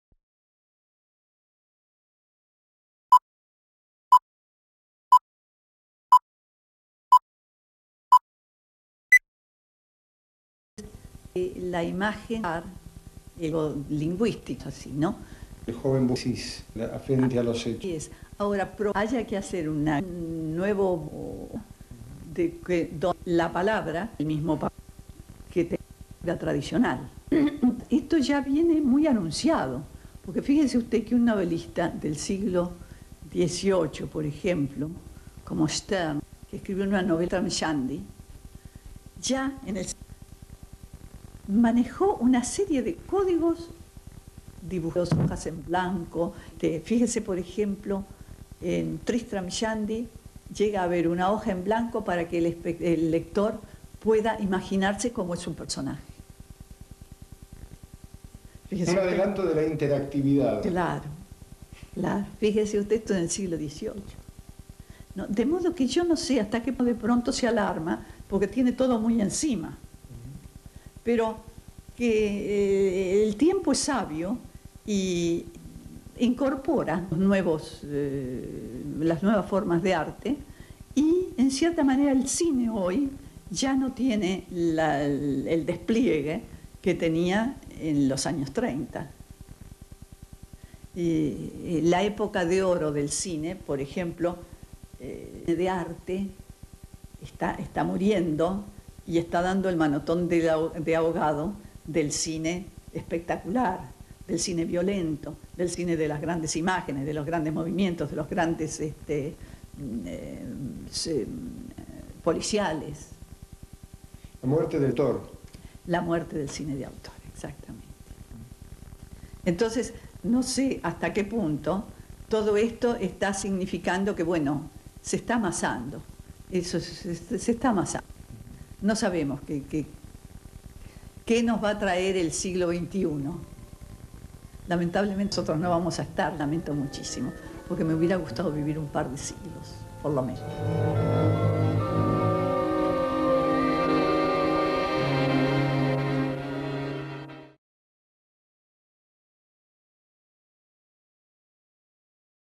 DiFilm - Entrevista a Mirta Arlt (1996)
Fecha: 26/12/1996
Duración: 2 minutos 33 segundos
Código: V-01830

Electra Mirta Arlt (n. 25 de enero de 1923) es una escritora, traductora, profesora, guionista e investigadora argentina, especializada en teatro. Es la hija del célebre escritor Roberto Arlt.